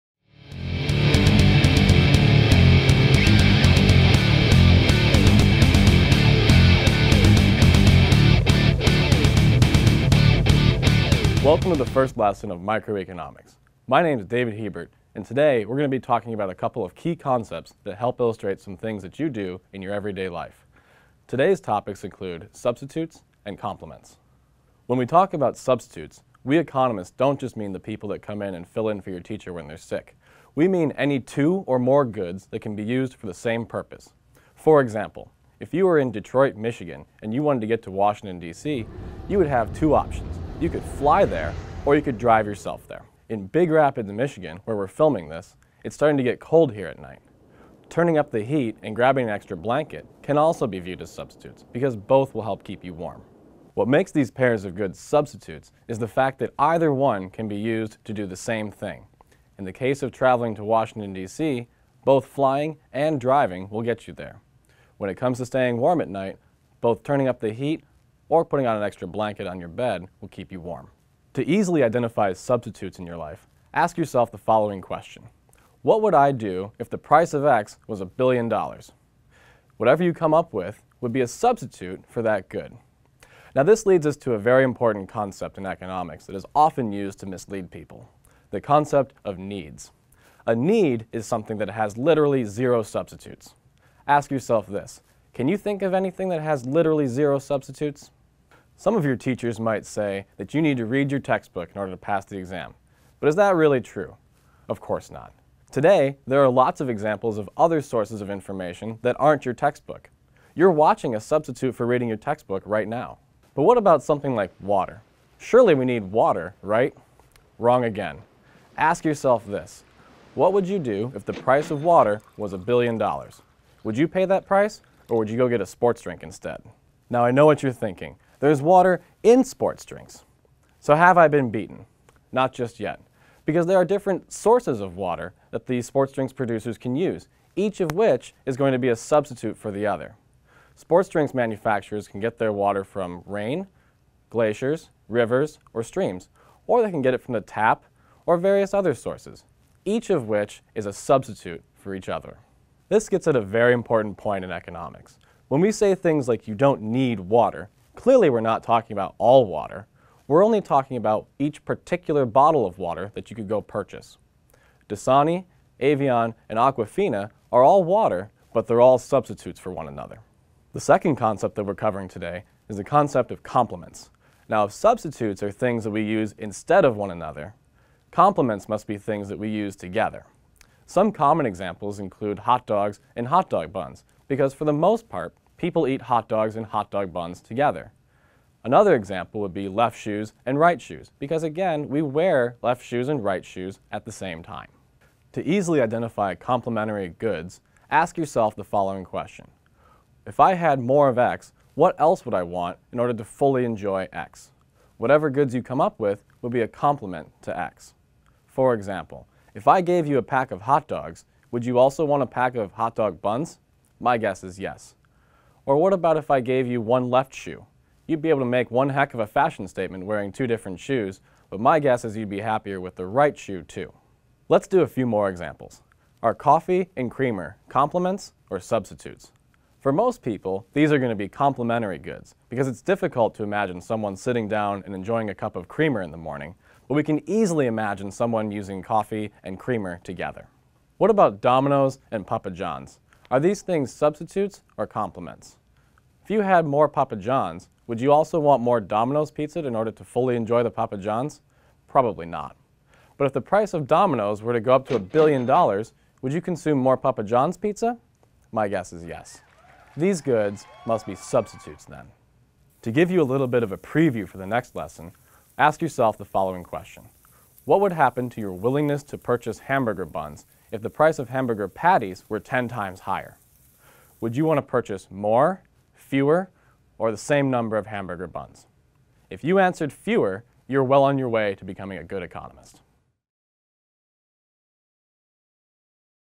Economics: Substitutes and Complements
Video produced by Media Production for economics professor
Dr. David Hebert at Ferris State University to be used on his YouTube channel to assist his and other econ students in various economic topics.